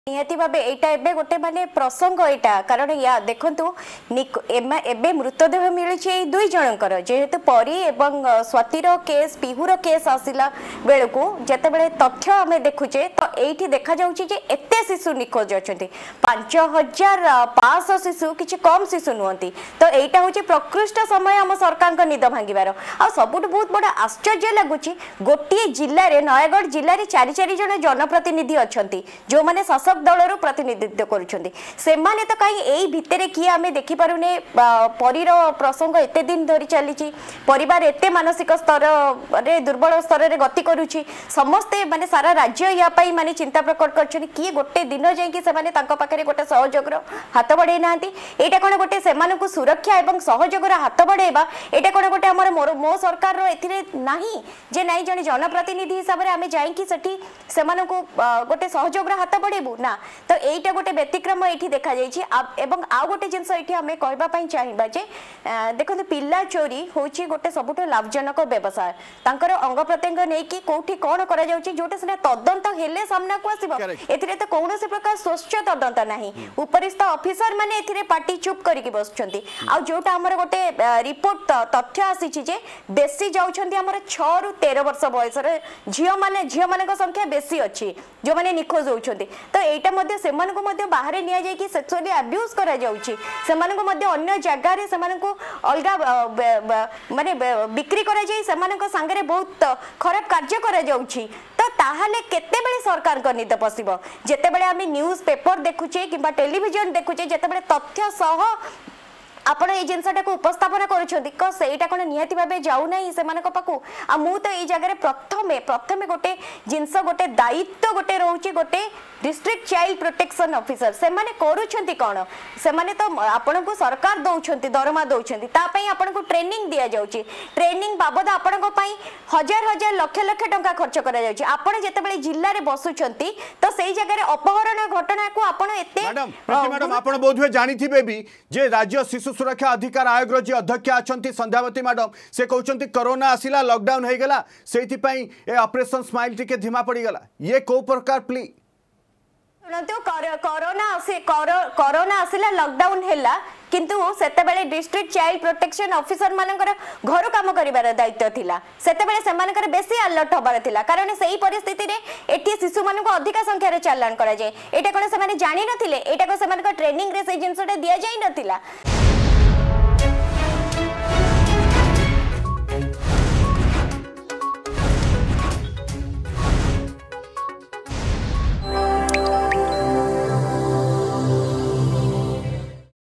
ନୟାଗଡ଼ ଜିଲ୍ଲାରେ ଦୁଇଟି କେସ ହେଲାଣି
ନୟାଗଡ଼ ଜିଲ୍ଲାରେ ଦୁଇଟି କେସ ହେଲାଣି ହେଲେ ଜିଲ୍ଲାର 4ଜଣ ଜନପ୍ରତିନିଧି ଯିଏ କି ଶାସକ ଦଳର ପ୍ରତିନିଧିତ୍ବ କରୁଛନ୍ତି ସେମାନେ କାହିଁକି ମୁହଁ ଖୋଲୁନାହାନ୍ତି : ପ୍ରୀତି ଧଳ ସମାଜସେବୀ